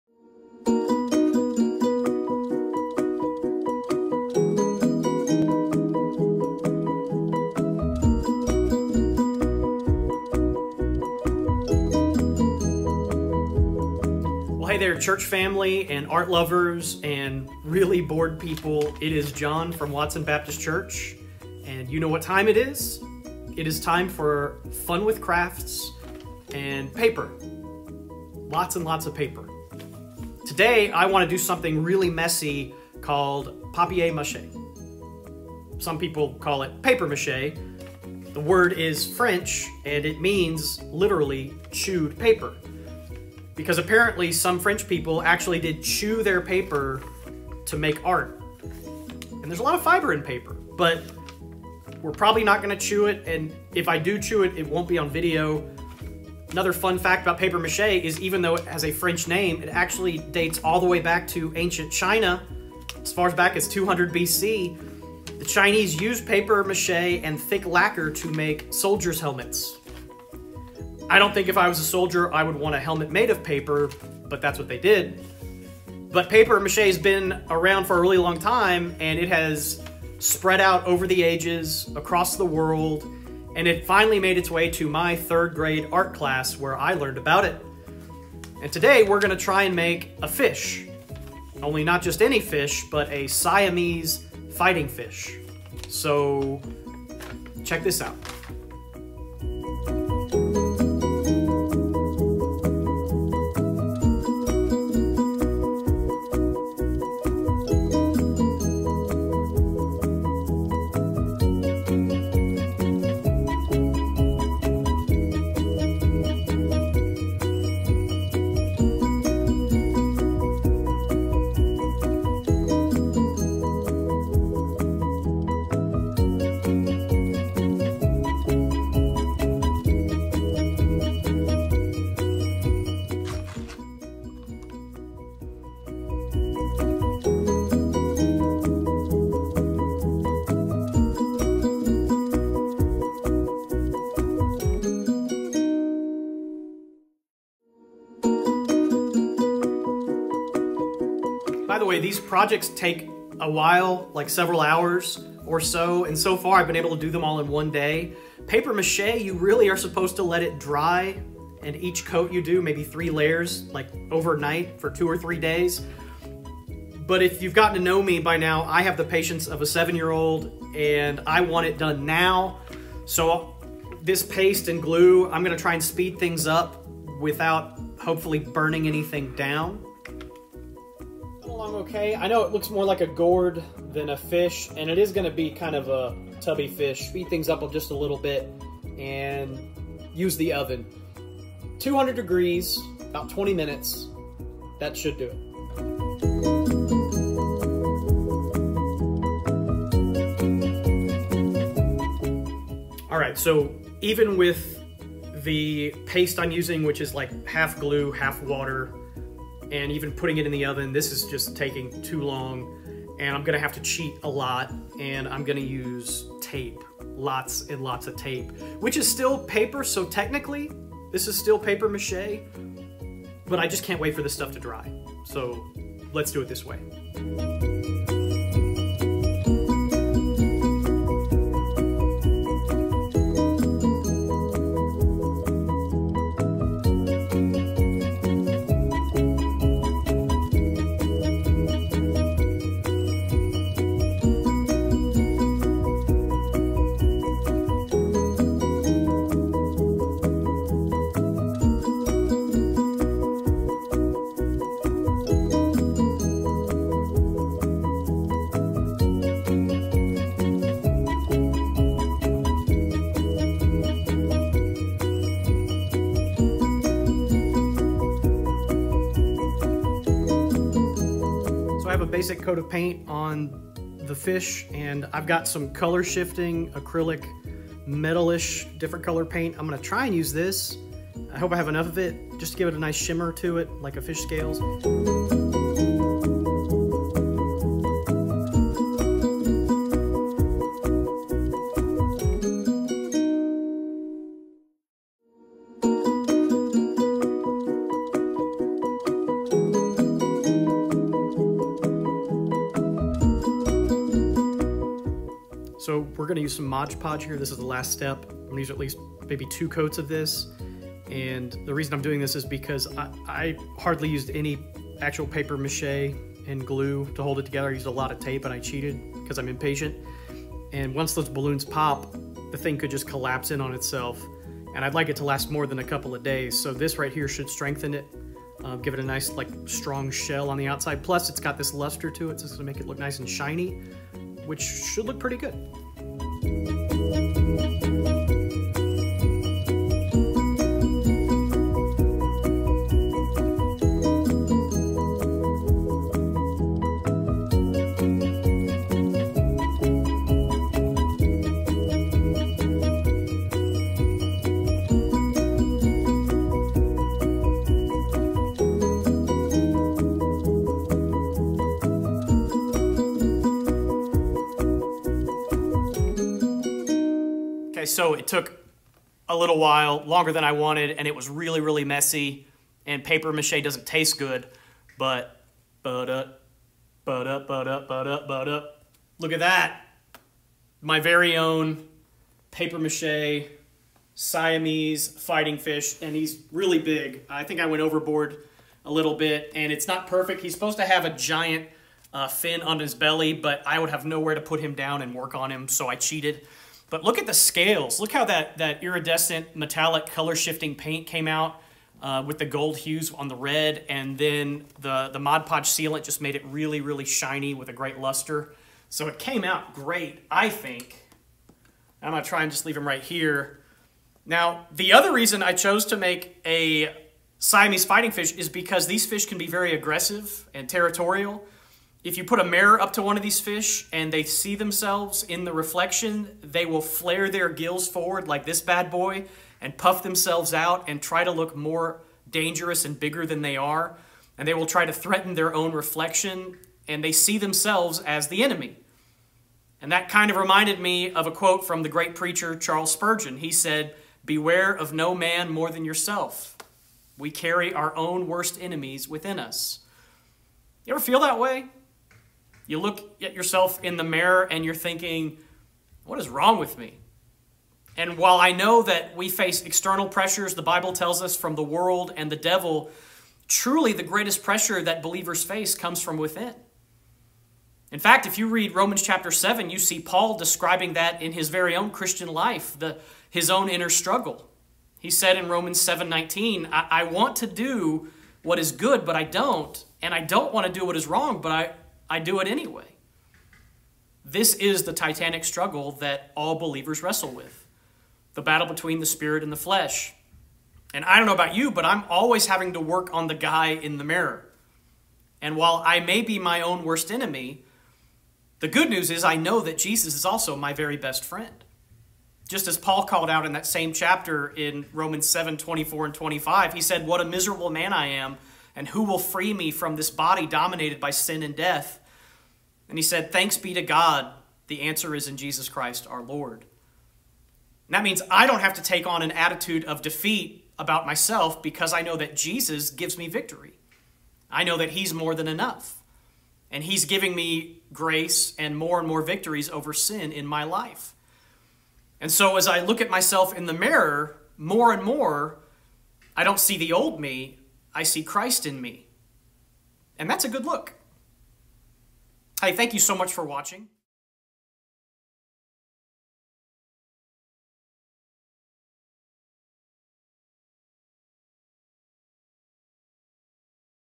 “Taper Mache,” Fun with Paper
Bored pastor connects arts and crafts with Scripture.  In this video he uses paper mache  (or maybe “taper” mache) to make a Siamese Fighting Fish.